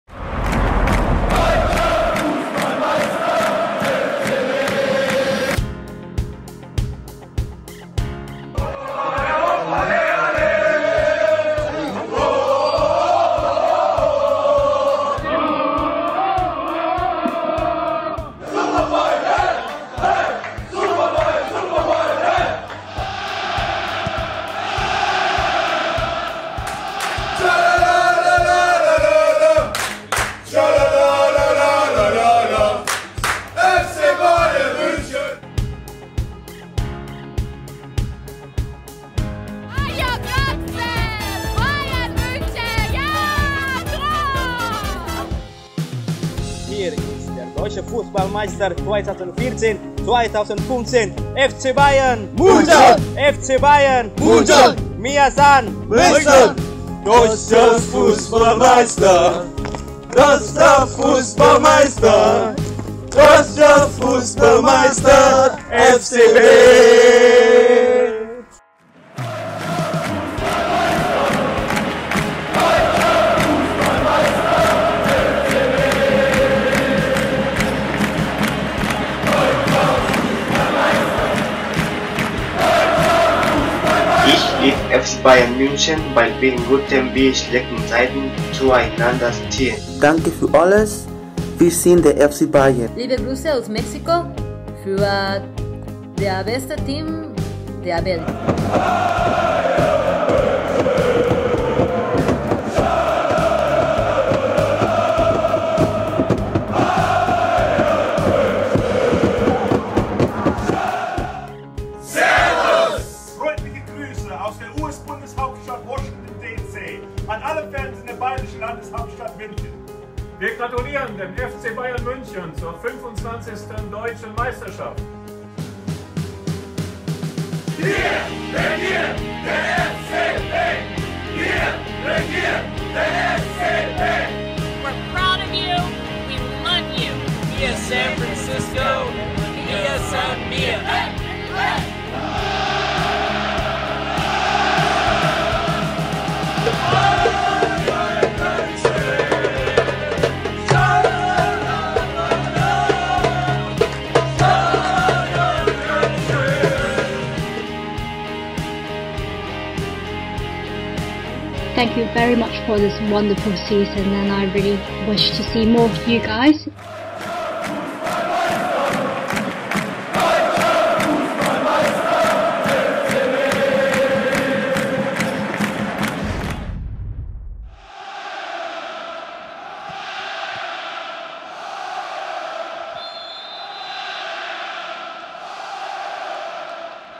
Mia san CHAMPIONS - Fans worldwide congratulate FC Bayern
FC Bayern-Fans weltweit gratulieren dem FC Bayern zur 25. Deutschen Meisterschaft.

FC Bayern fans worldwide congratulate FC Bayern on winning their 25th German Championship.